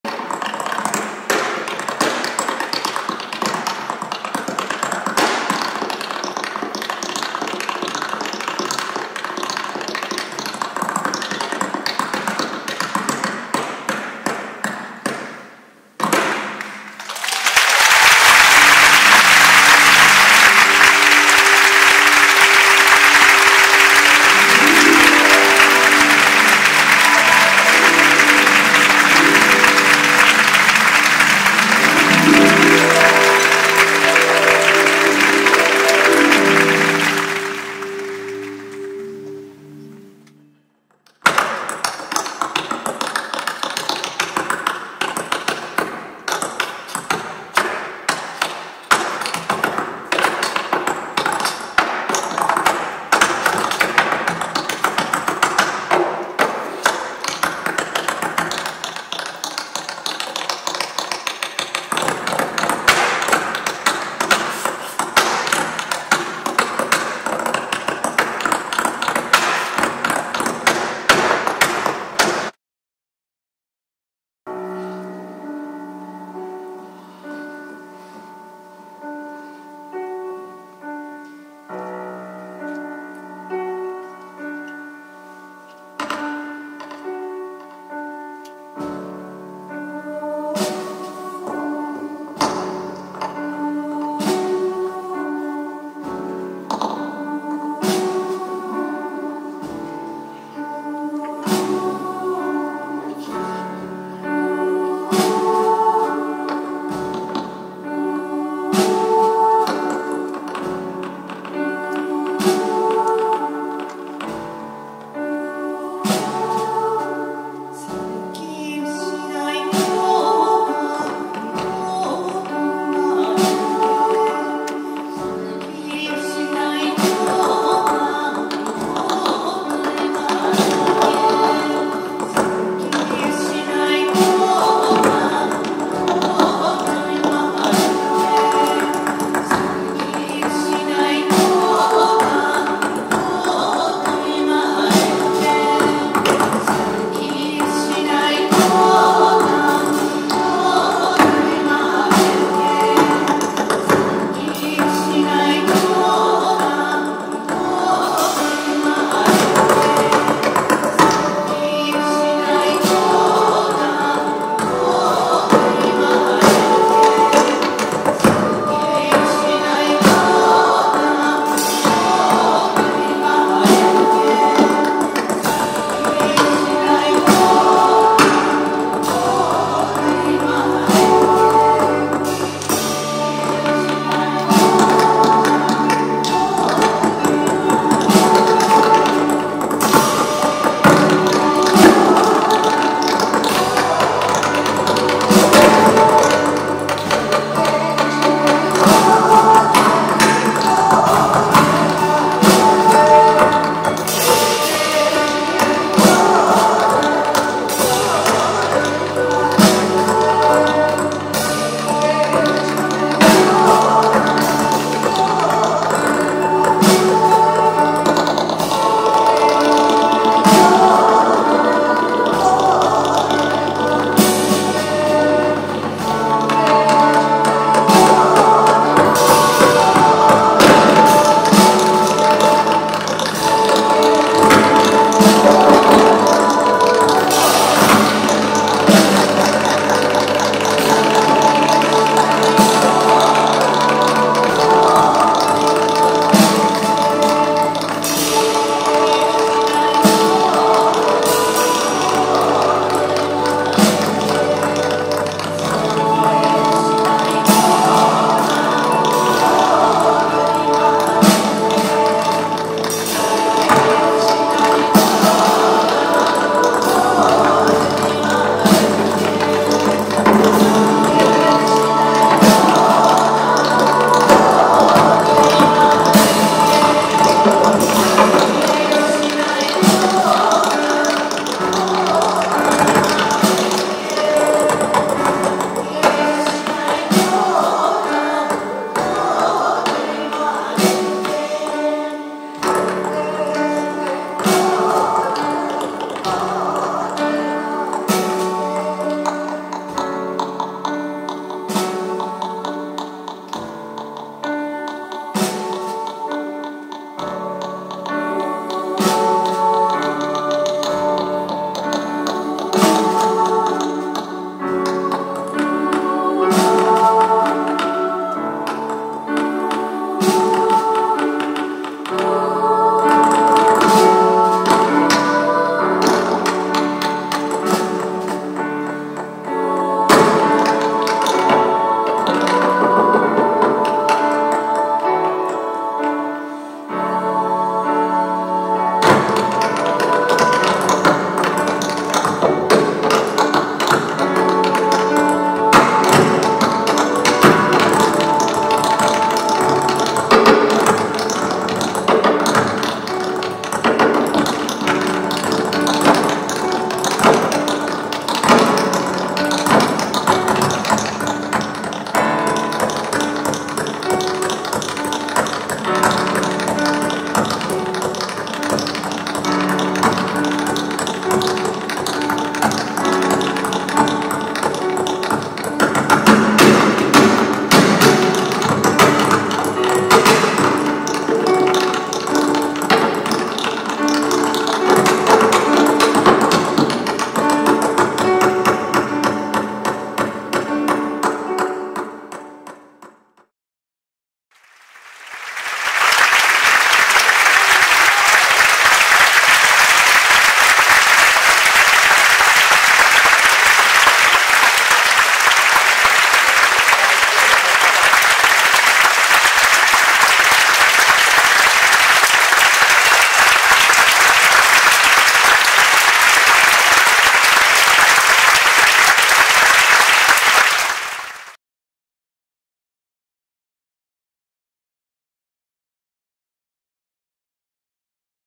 Kazunori Kumagai 'Voice' - Highlights
Kazunori Kumagai 'Voice'
Date: May 28, 2022
Venue: Bunkamura Orchard Hall

Tap Dancer: Kazunori Kumagai

Guest Vocal:
Chitose Hajime
AKAN AINU Traditional Singer
 Kikuko Hikawa
 Eiko Yamamoto
 Midori Toko
 Kayo Watanabe
 Emi Toko
 Fukiko Goukon

Bass/Piano: Masato Suzuki
Harp: Aya Horigome
Drums: Shinya Fukumori

Chorus: Miho Ota & U+TA
U+TA
Ai Kamano
Rie Nemoto
Yoko Komatsu
Miki Katsumasa

熊谷和徳『VOICE ｳﾞｫｲｽ』
2022年5月28日(土) Bunkamuraオーチャードホール

熊谷和徳

ヴォーカルゲスト：元ちとせ
アイヌ伝統歌の唄い手：
日川 キク子
山本 榮子
床 みどり
渡辺 かよ
床 絵美
郷右近 富貴子

ミュージシャン：
鈴木正人（ピアノ/ベース）
堀米綾（ハープ）
福盛進也（ドラムス）

コーラス：太田美帆 ＆ U+TA（ゆた）
U+TA
鎌野愛
根本理恵
小松陽子
勝政美紀